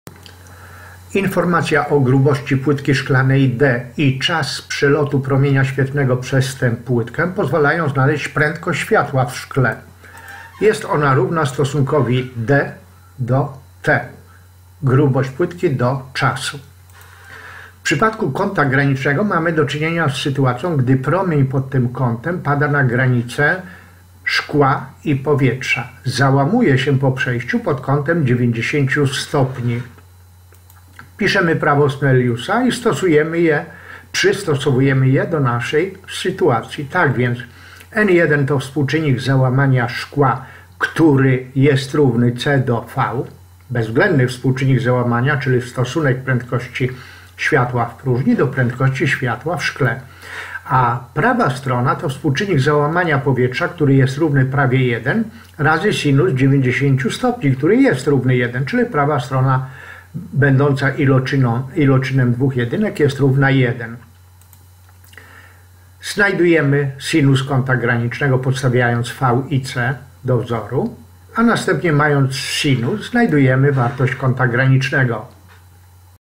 627. Fizykamax, optyka geometryczna, kąt graniczny, prawo załamania
Kąt graniczny. Stosujemy prawo załamania dla prostego kąta załamania. Korzystamy z definicji prędkości i współczynnika załamania. Prędkość światła w próżni